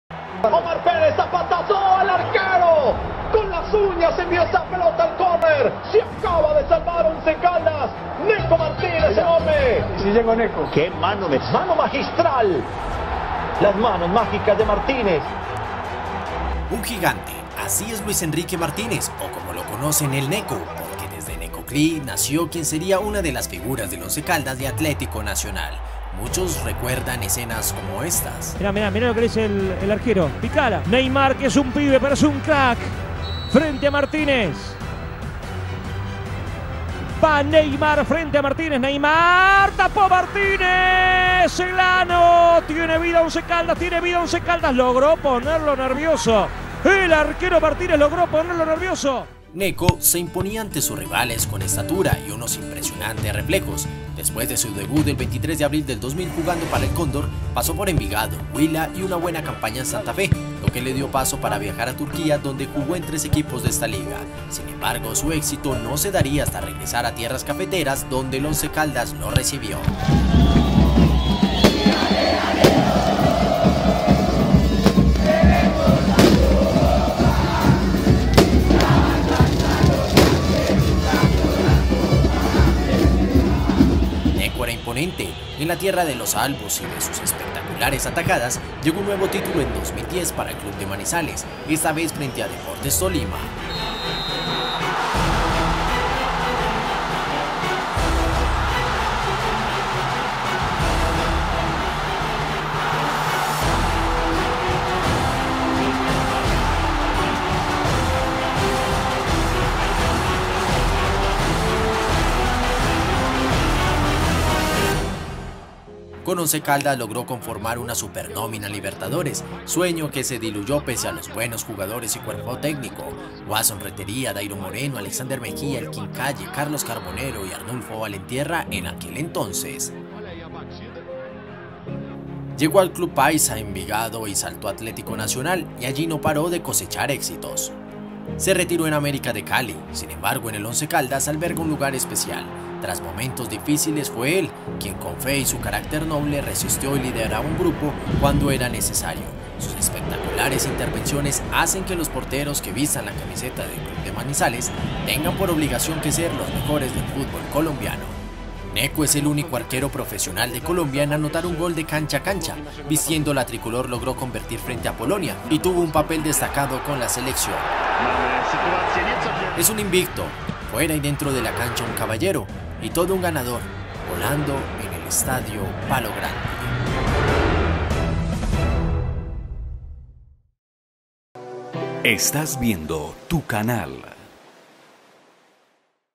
Invictos - Luis "Neco" Martínez
Nuestro Invicto hoy es un gigante, así es Luis Enrique "Neco" Martínez quien con sus espectaculares atajadas conquistó los seguidores del Once Caldas S.A.